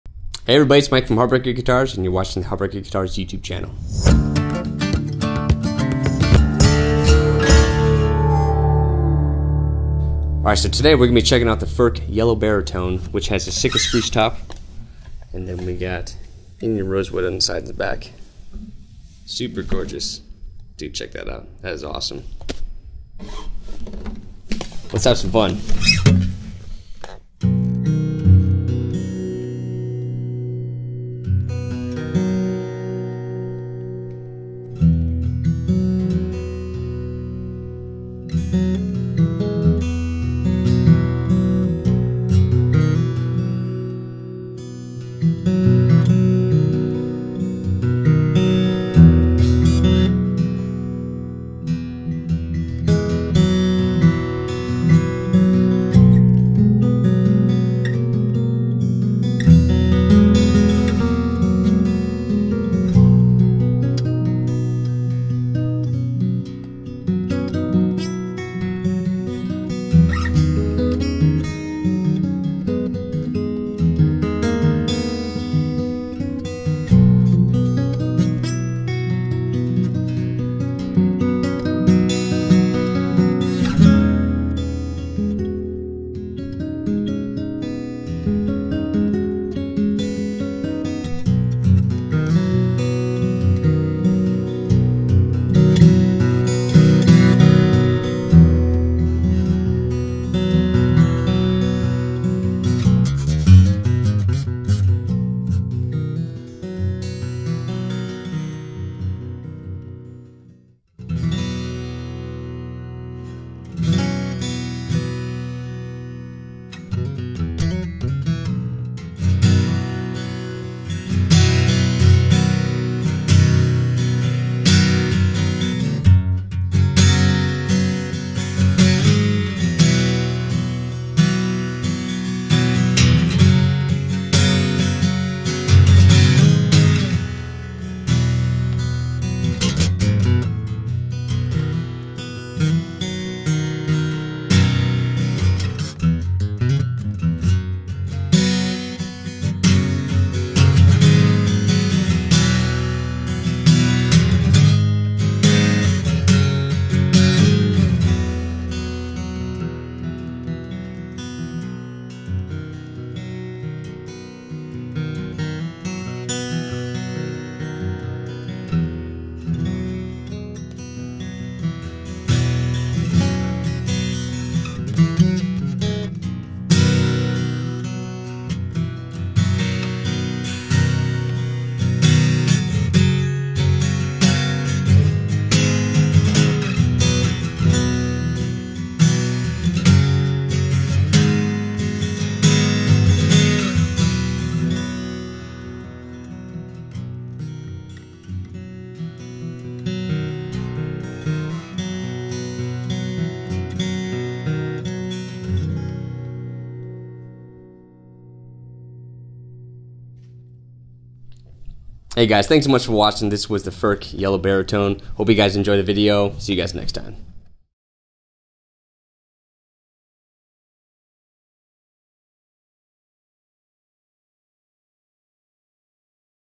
Furch Guitars - Yellow Series Baritone with Mike Romano | 4k Video
This Baritone guitar is a top quality instrument made from premium tonewoods. The soundboard is made from high grade Sitka Spruce, and the back and sides feature Indian Rosewood.

Heartbreaker Guitars - High end Acoustic Guitars